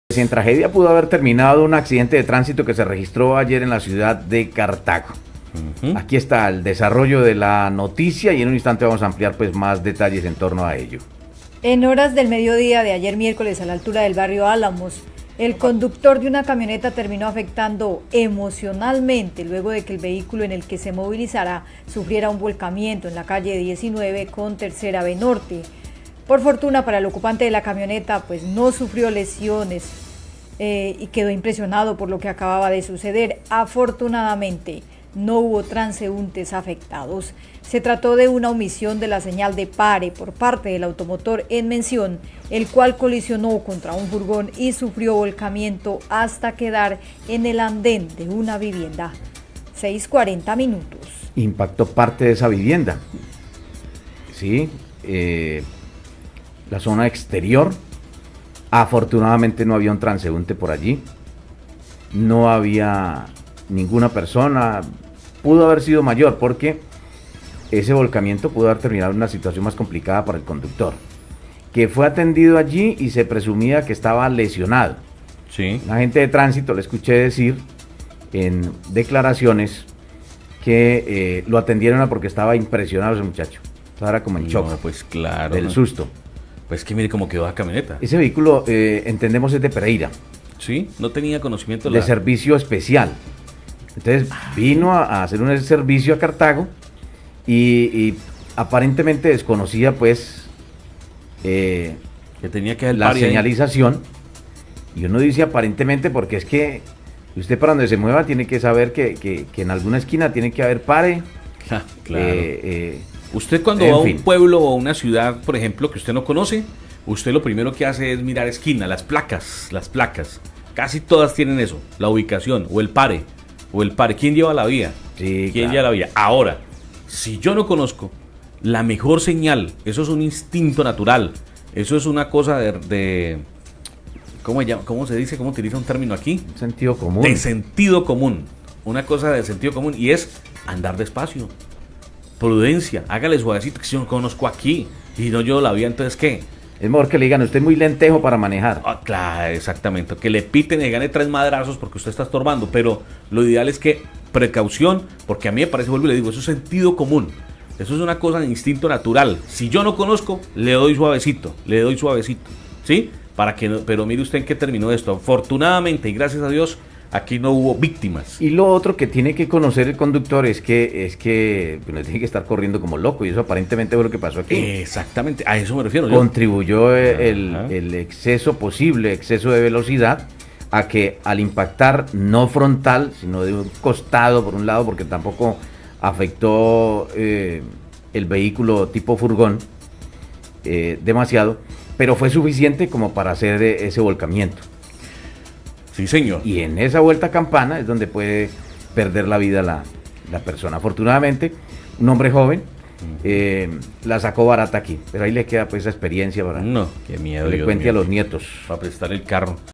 En tragedia pudo haber terminado un accidente de tránsito registrado en el barrio Alamos
En horas del mediodía de este miércoles, a la altura del barrio Álamos, el conductor de una camioneta terminó afectado emocionalmente luego de que el vehículo en el que se movilizaba sufriera volcamiento en la calle 19 con 3B Norte.